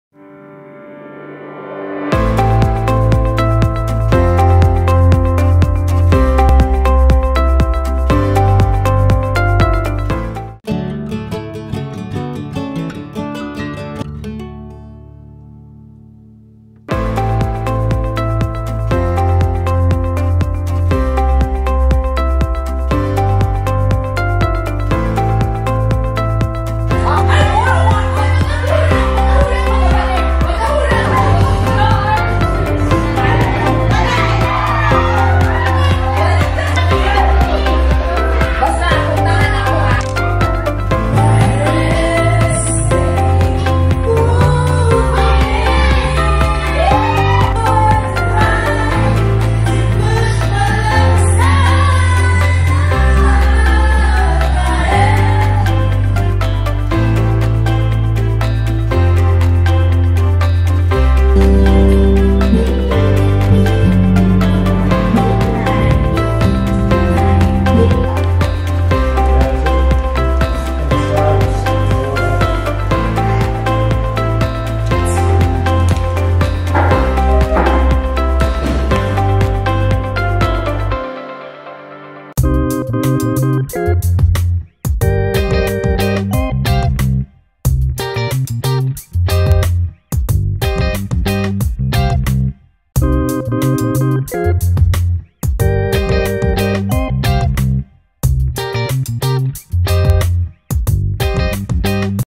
Ein kleiner Einblick in unser Weihnachten auf den Philippinen zum ersten mal nach auswandern!
Wir hatten ein schönes Weihnachten. Zum ersten mal in den Philipinnen zusammen als ehe paar. Und das zweite mal insgesamt. Allerdings haben wir das letzte mal unsere Hochzeit vorbereite und Weihnachten ging mehr oder weniger an uns vorbei.

Wir haben aber viele Sachen aus Deutschland vermisst. Weihnachten im warmen ist halt wieder gewöhnungsbedürftig. Es sind halt die kleinen Sachen die Auswandern nicht so einfach machen.